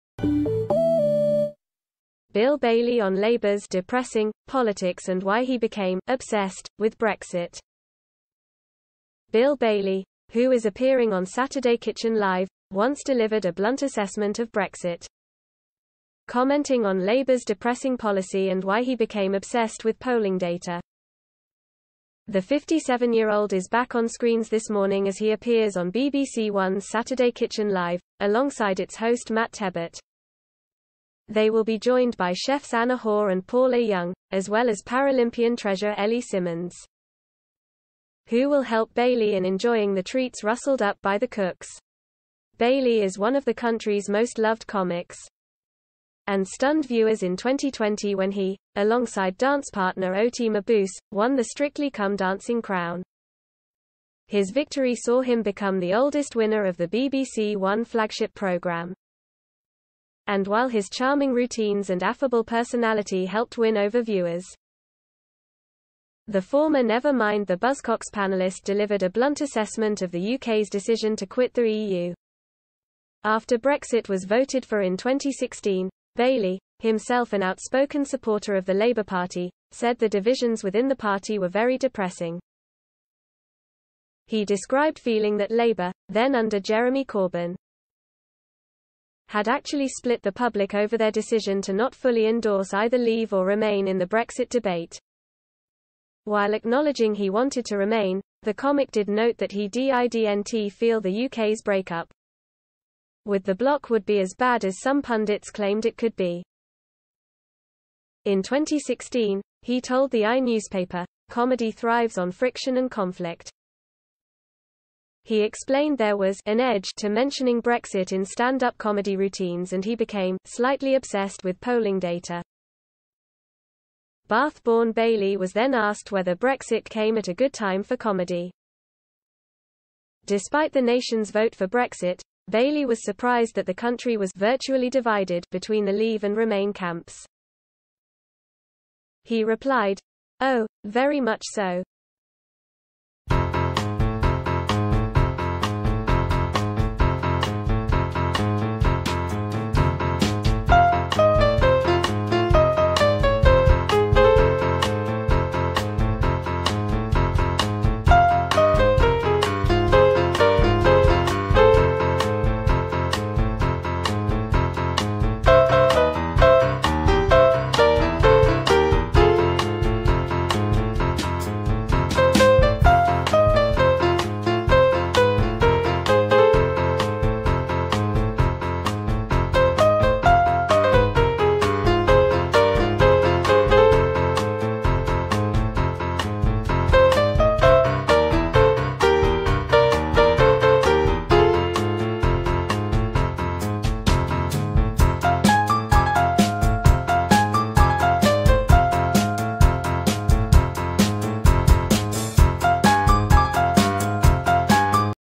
Bill Bailey on Labour's ‘depressing’ poIitics and why he became ‘obsessed’ with Brexit
BILL BAILEY, who is appearing on Saturday Kitchen Live, once delivered a blunt assessment of Brexit.